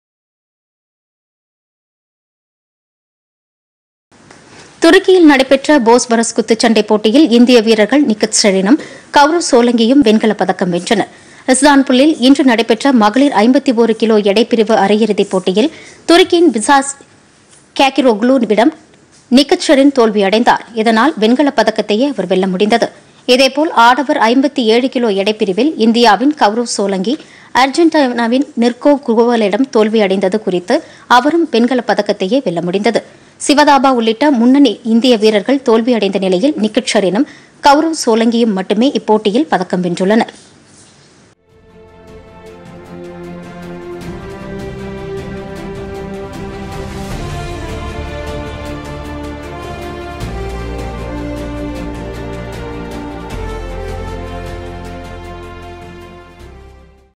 போஸ்போரஸ் குத்துச்சண்டை போட்டிPodhigaiTamilNews
துருக்கியில் நடைபெற்ற போஸ்போரஸ் குத்துச்சண்டை போட்டி - வெண்கலப் பதக்கம் வென்றனர் இந்தியாவின் நிக்கத் ஸரீன், கவுரவ் சோலங்கி.